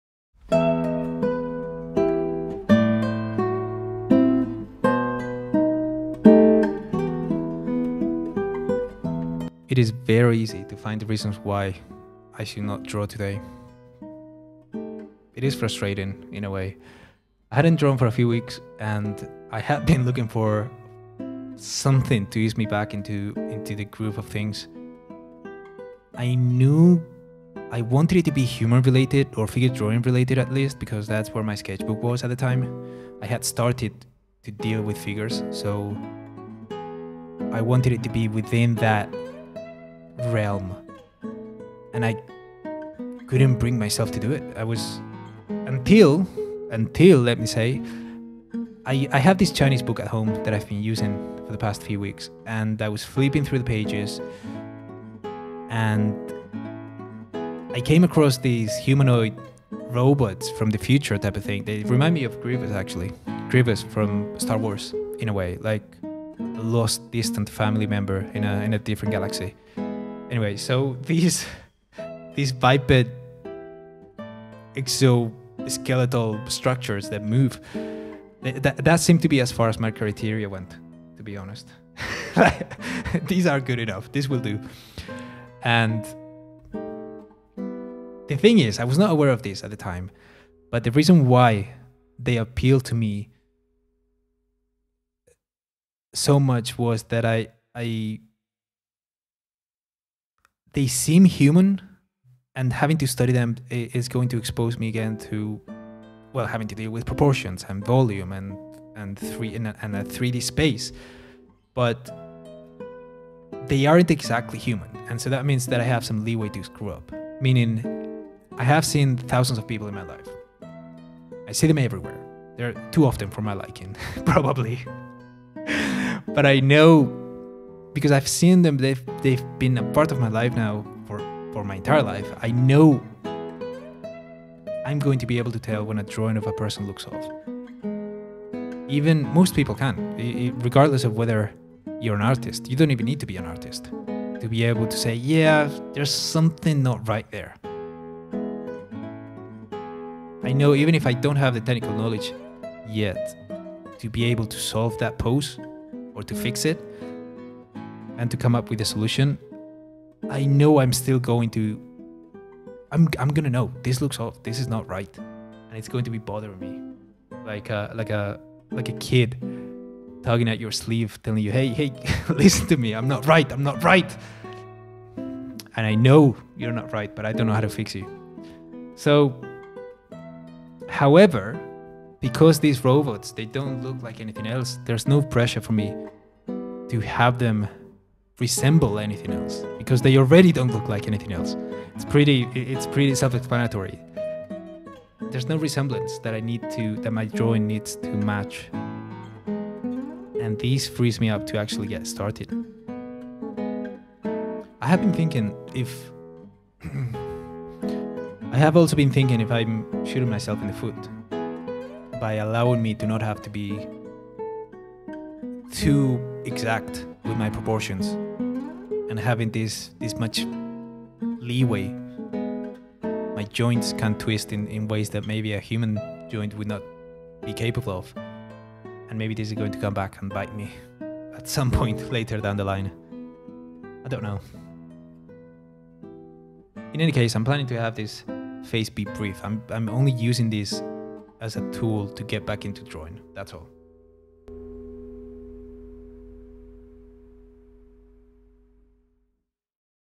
4. TRAINING. On learning figure drawing from robots - or tricking your inner compass.
Training block. On learning figure drawing from robots - or tricking your inner compass.

I have been drawing a lot these past couple of weeks. I have started to study and learning figure drawing. Using humanoid robots for it.